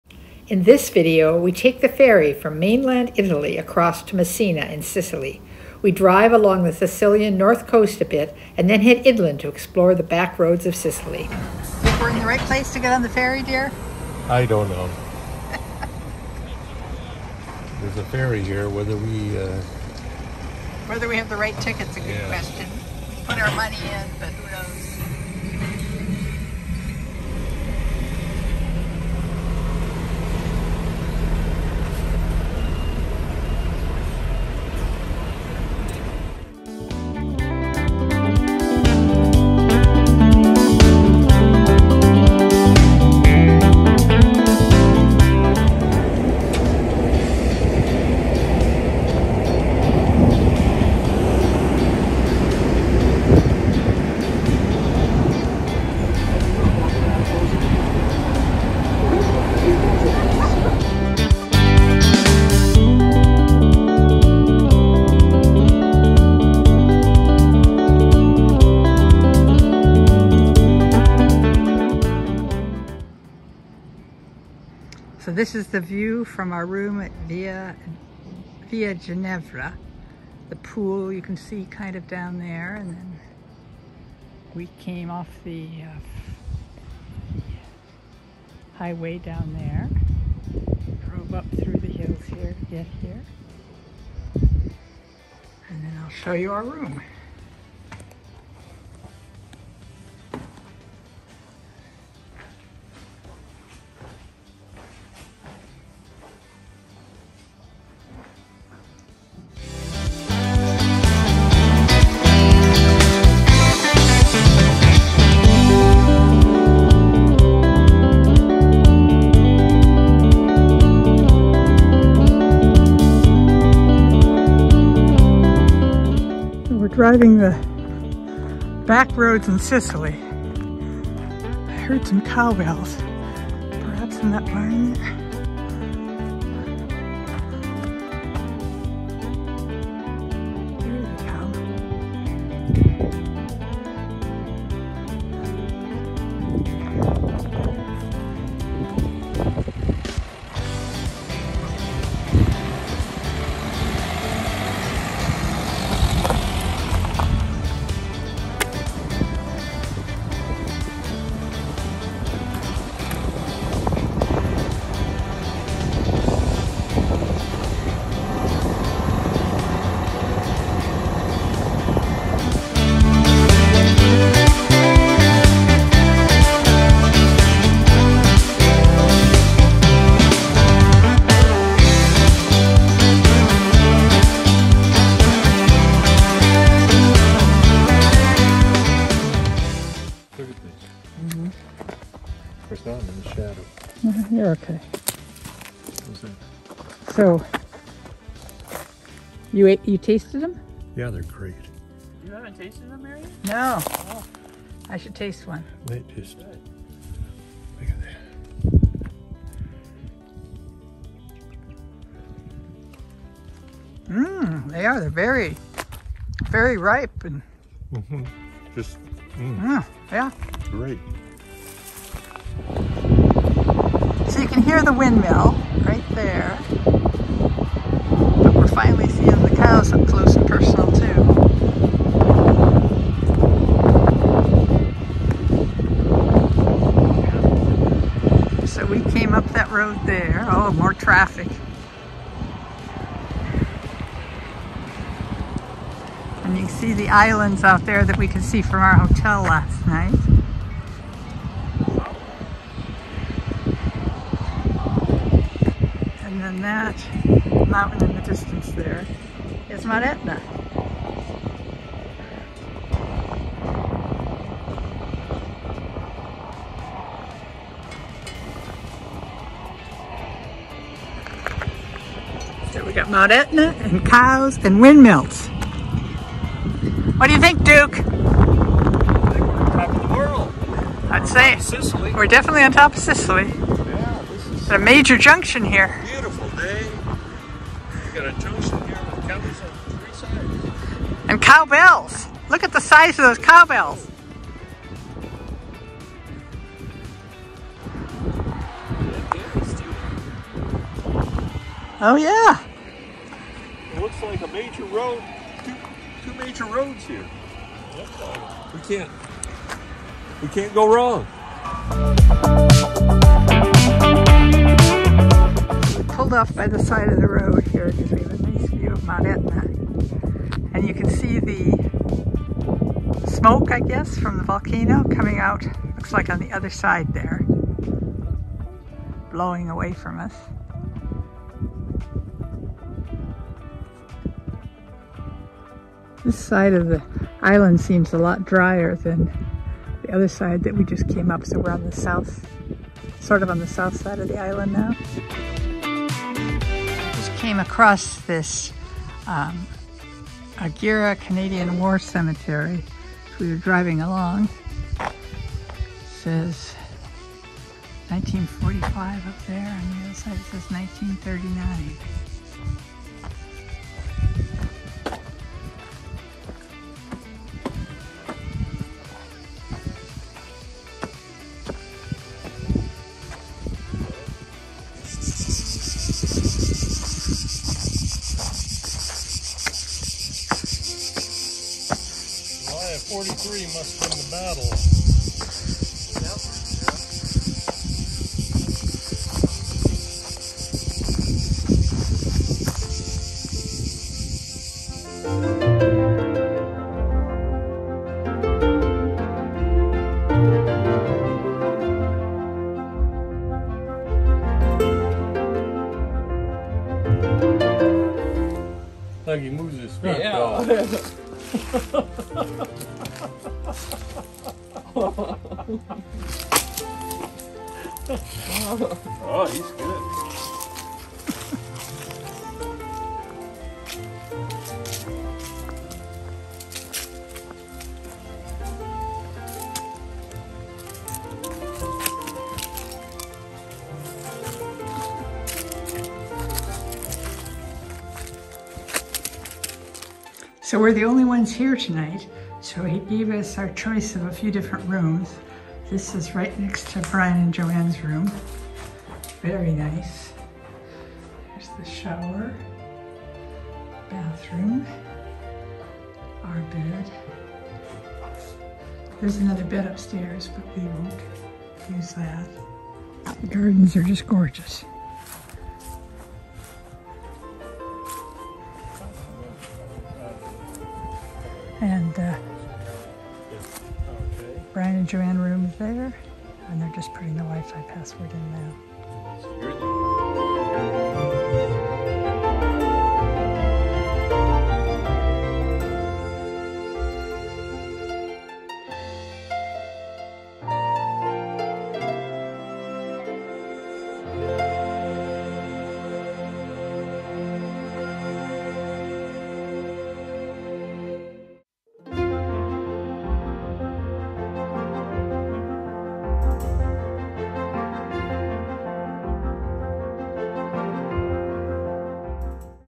Driving the Backroads of Sicily - We take the Ferry to Sicily and Drive through Central Sicily
We are Duke and Marion, Boomers Without Borders. In this video we are in Italy in mid September, 2022. We took the ferry from the tip of mainland Italy to Messina in Sicily.

From Messina we drove to Ficcara and stayed at Villa Genevra. Then we drove into Cental Sicily. We ate wild raspberries, saw Mount Etna, and saw lots of cows. We visited Agir Canadian War Cemetery on our way to Pergusa. We spent the night at Villa Pastorelli in Pergusa.

I'll get $15 off of my bill if you use this link to sign up!